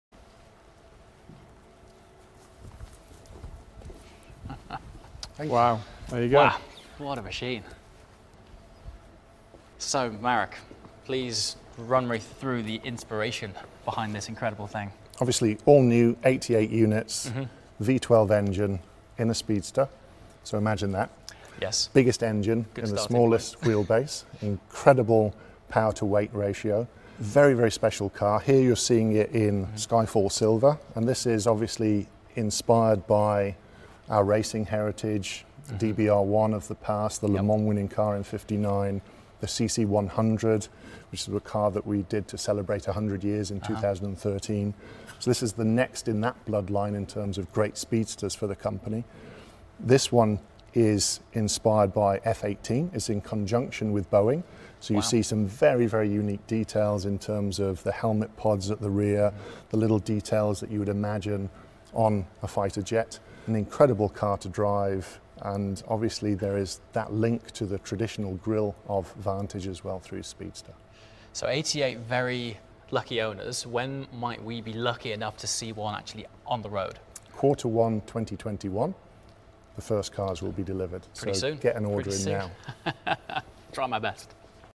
V12 Speedster Reveal with Aston Martin Design Boss Marek Reichman
AstonMartin's chief creative officer Marek Reichman tells us about the design inspiration for the new V12 Speedster. More